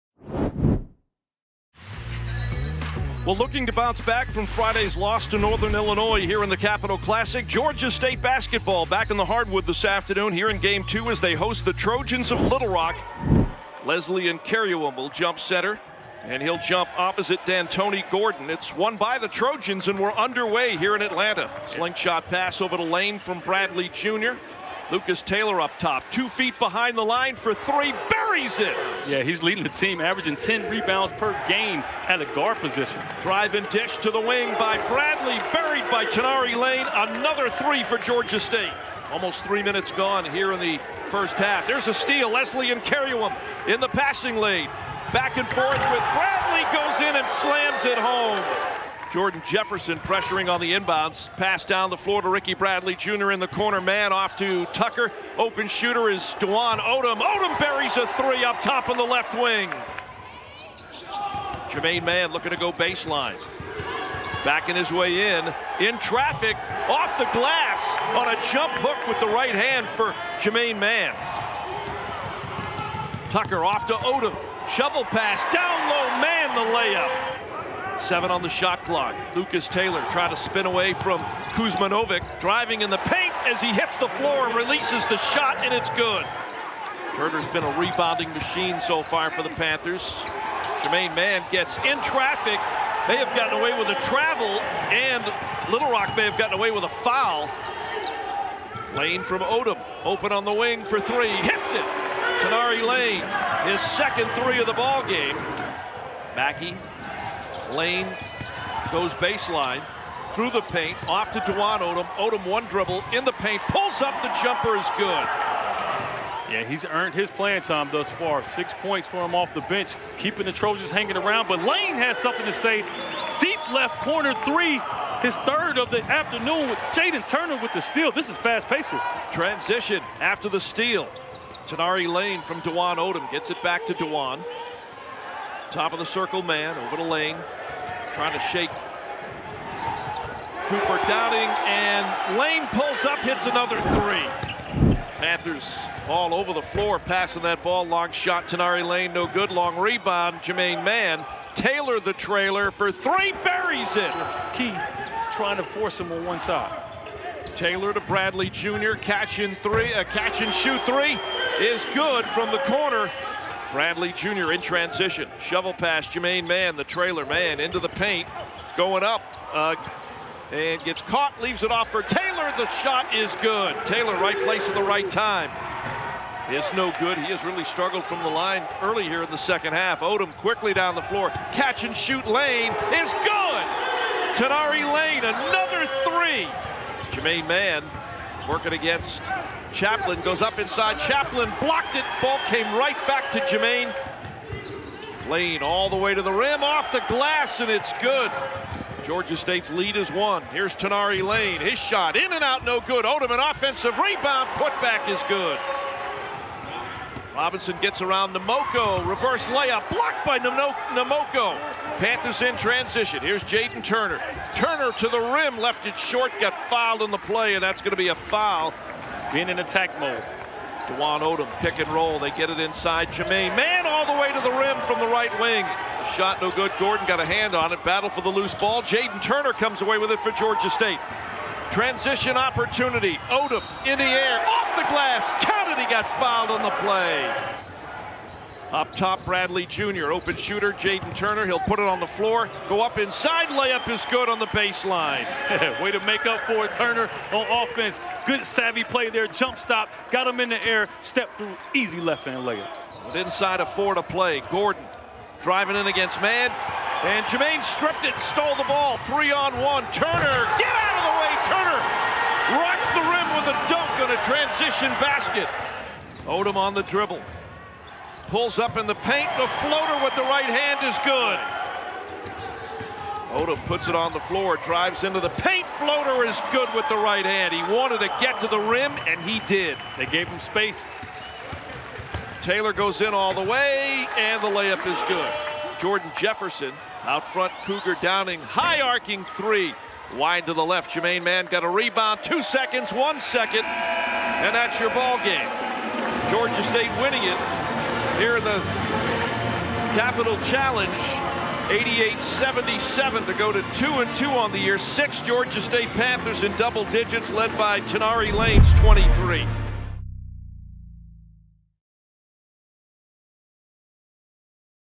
HIGHLIGHTS: GSU vs. Little Rock | Men's Basketball
Georgia State vs. Little Rock
November 19, 2023 | Atlanta, GA
Men's Basketball

Presented by Georgia Power